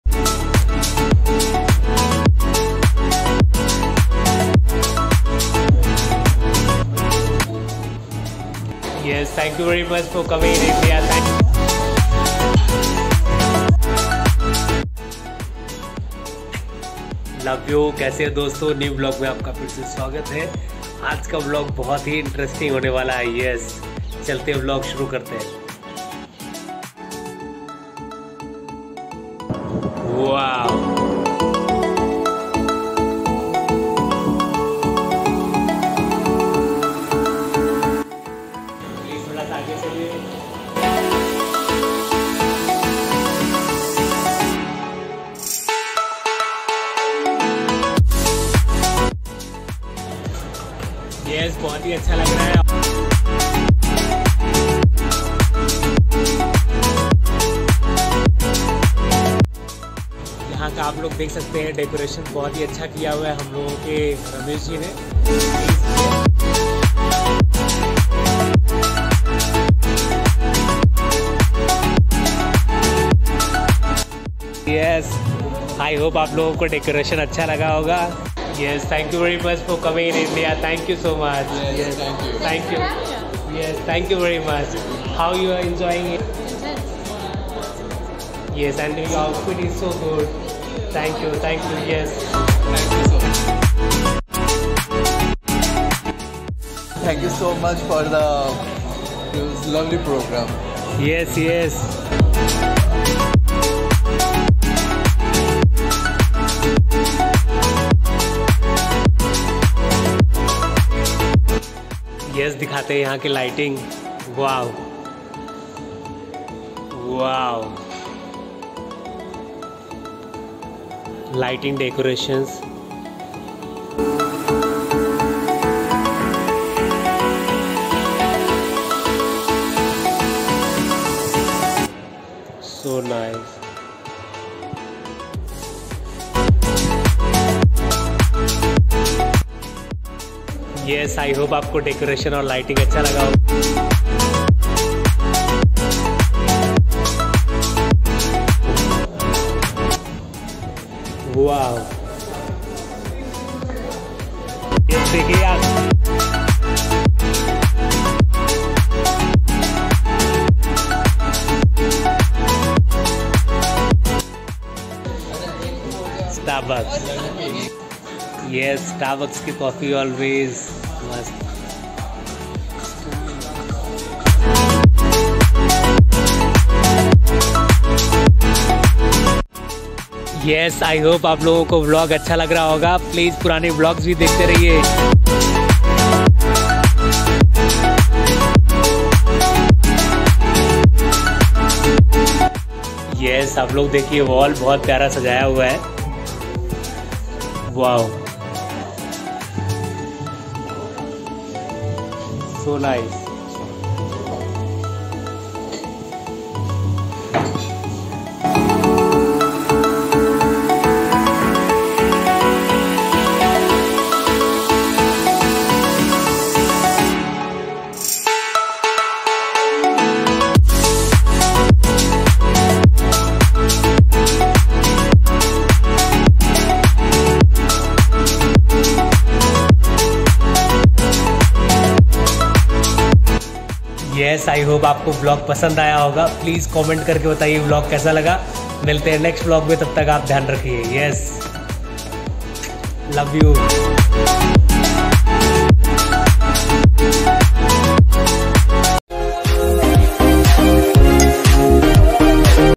A Day In The Life 😍 जीवन में एक दिन 🎉 सेलिब्रेशन व्लॉग
Shivanatics | Shivendra Singh Rajput

Hello Everyone This is Shivendra Singh Rajput. Mr. Mumbai ,Naagin 3 ,Ye Rishta Kya Kahlata hai fame actor.I spent wholeday in a celebration.Many more things happened in a vlog.They enjoyed a lot.coming more things.keep watching old vlogs also my friends.Learnt many things & enjoyed a lot.Really felt very good.watch atleast once my vlog!.Thank you very much for watching!🙏🏻